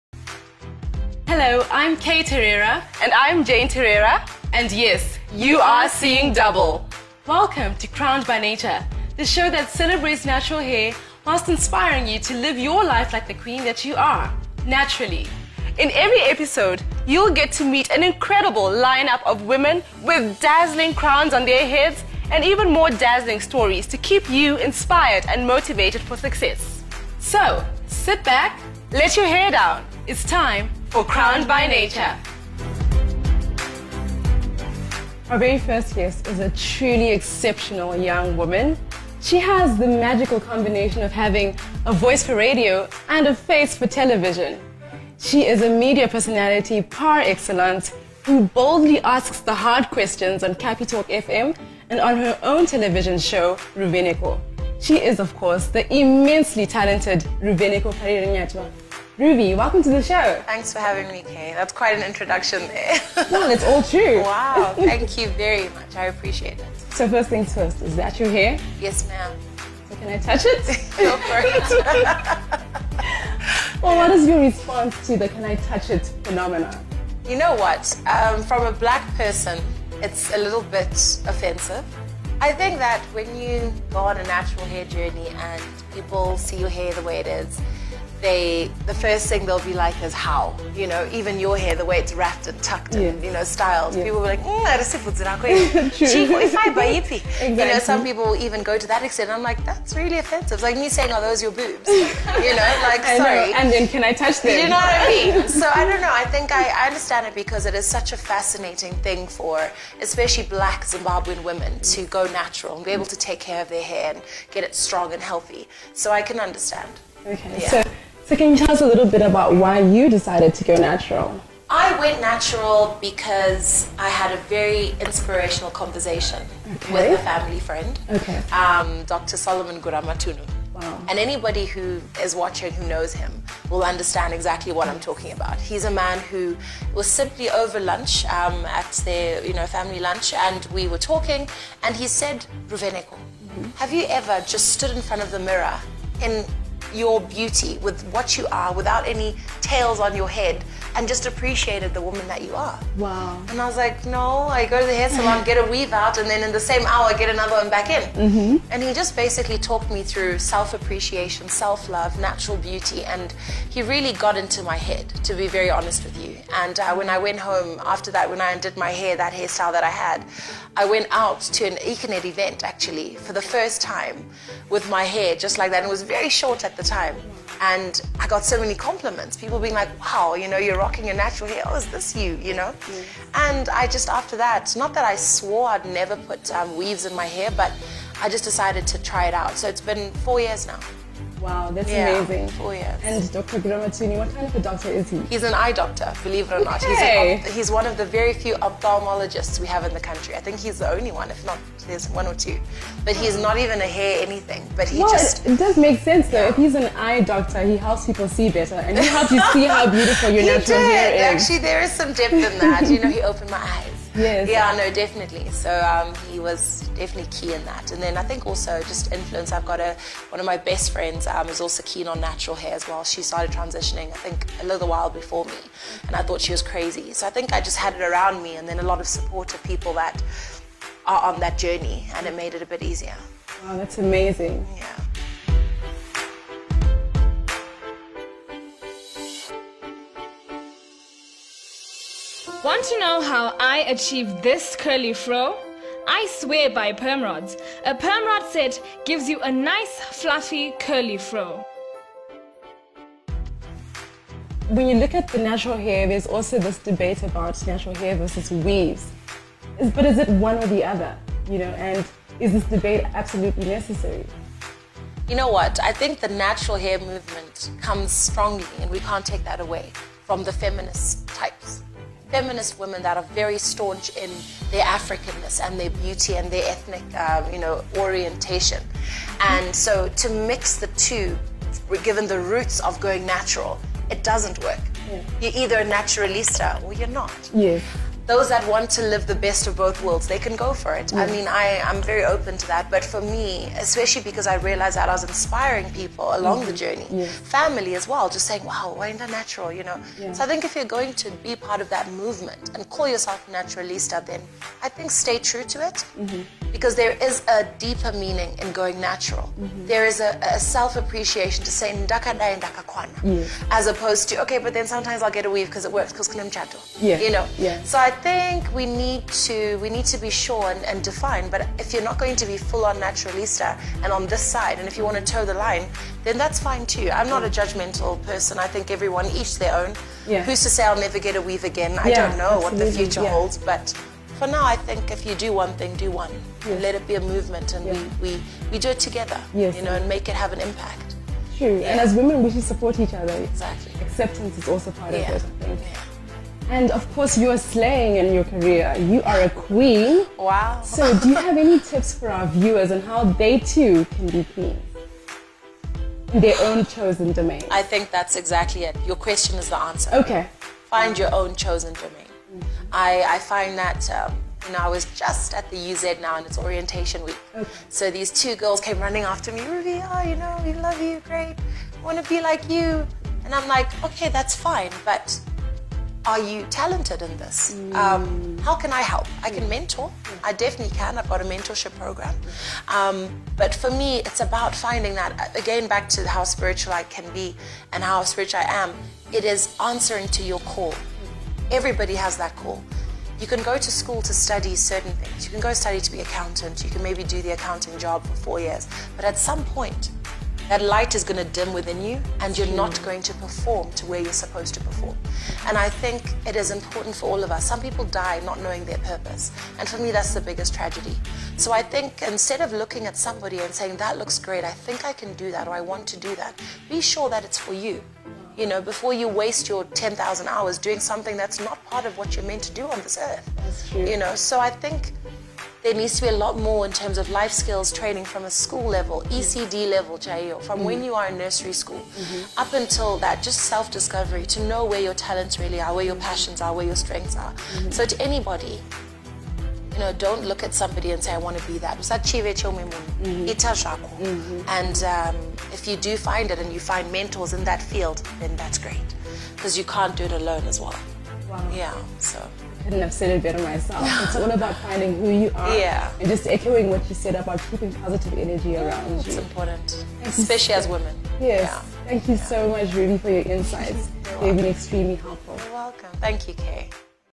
Jayne & Kay Crowned by Nature: Ruvheneko Parirenyatwa
Jayne and Kay Terera speak to media personality Ruvheneko Parirenyatwa.